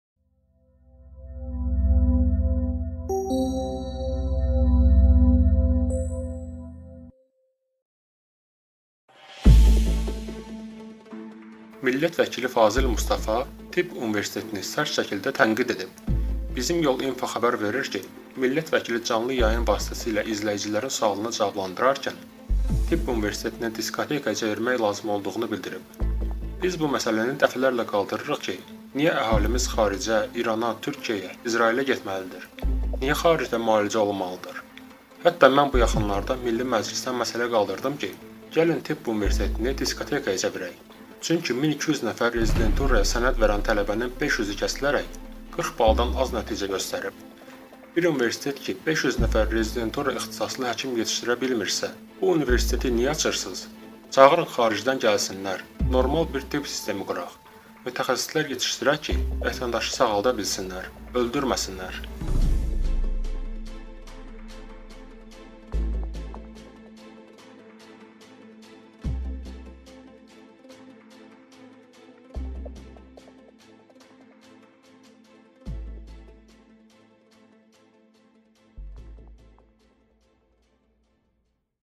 Fazil Mustafa: “Məclisdə məsələ qaldırmışam ki, Tibb Universitetini diskotekaya çevirək”
Millət vəkili Fazil Mustafa Tibb Universitetini sərt şəkildə tənqid edib.

Bizimyol.info xəbər verir ki, millət vəkili canlı yayın vasitəsilə izləyicilərin sualını cavablandırarkən Tibb Universitetinin diskotekaya çevirmək lazım olduğunu bildirib:

“Biz bu məsələni dəfələrlə qaldırırıq ki, niyə əhalimiz xaricə, İrana, Türkiyəyə, İsrailə getməlidir, niyə xaricdə müalicə olunmalıdır? Hətta mən bu yaxında Milli Məclisdə məsələ qaldırdım ki, gəlin tibb universitetini diskotekaya çevirək. Çünki 1200 nəfər rezidenturaya sənəd verən tələbən 500-ü kəsilərək 40 baldan az nəticə göstərib. Bir universitet ki, 500 nəfər rezidenturaya ixtisaslı həkim yetişdirə bilmirsə, bu universiteti niyə açırsınız? Çağırın xaricdən gəlsinlər, normal bir tibb sistemi quraq, mütəxəsislər yetişdirək ki, vətəndaşı sağalda bilsinlər, öldürməsinlər”.